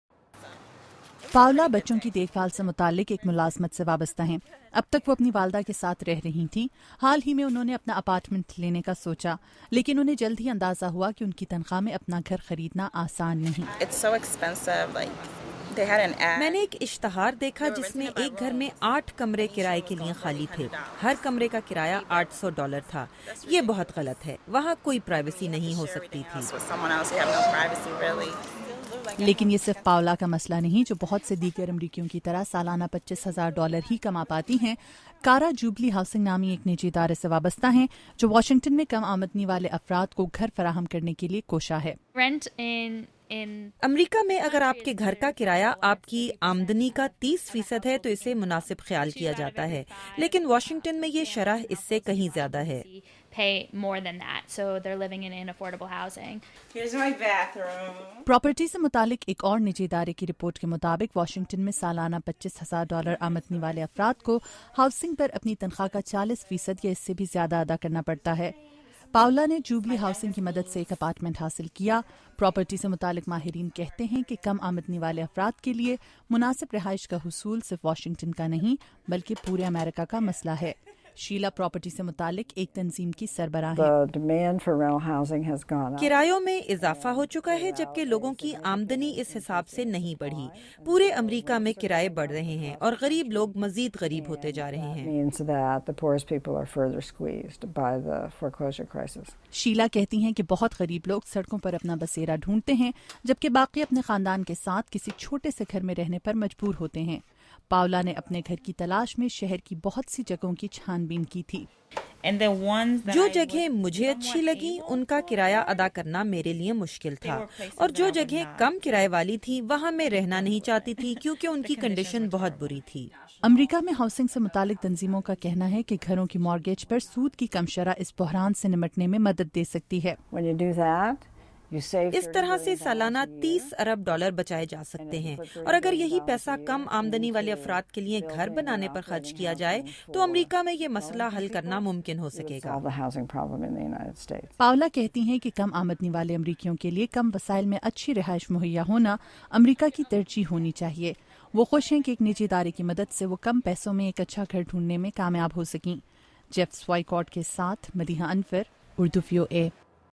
Housing Crisis- Madeeha Anwar/Raza Naqvi- Urdu VOA
Housing advocates say affordable housing is growing scarce in the United States with fewer people able to own their own homes and rents rising. This report tells about private efforts in the nation's capital to help people find safe, clean places to live.